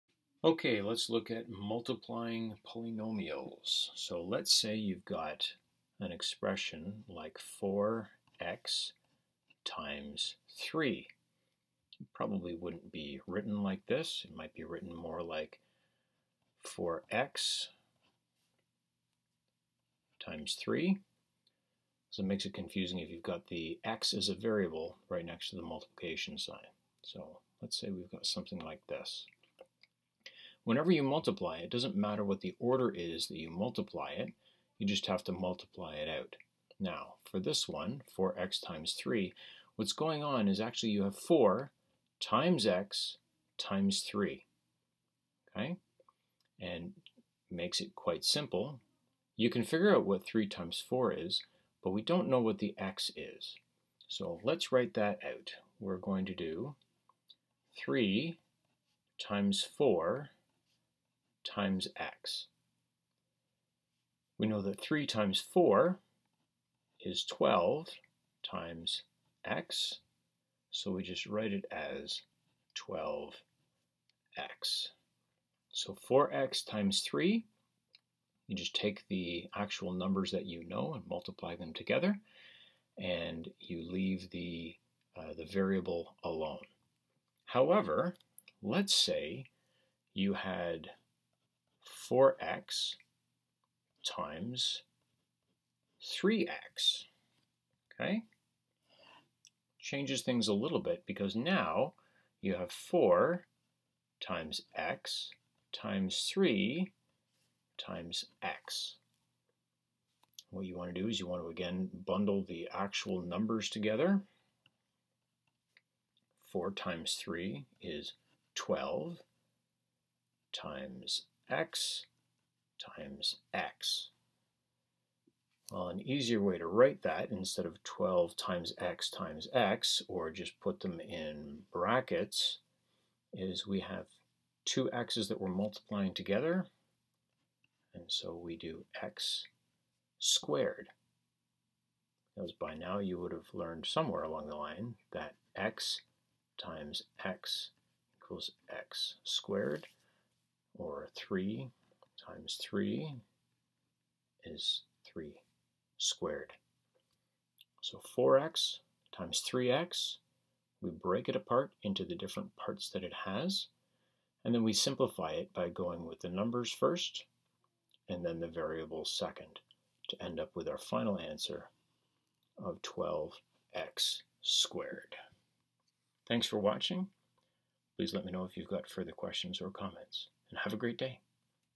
Multiplying Simple Polynomials
Forget how to multiply simple polynomials, like 5x times 6?  Spend three minutes watching this!  I hope it helps.